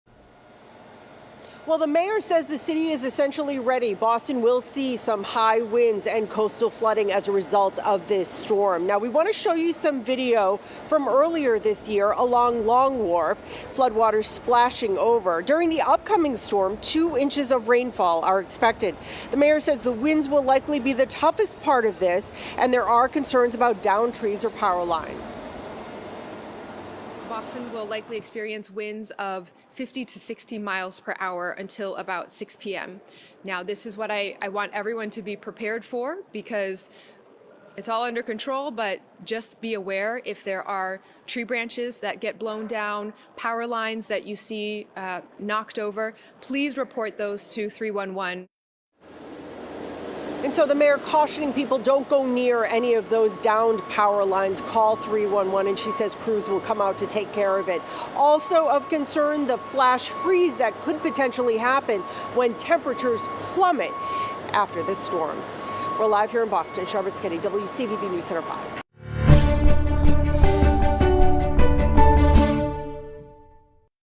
Boston prepares for high winds, coastal flooding as storm approaches
Boston Mayor Michelle Wu is warning residents of gusty winds and heavy rain as the storm moves in late Thursday night.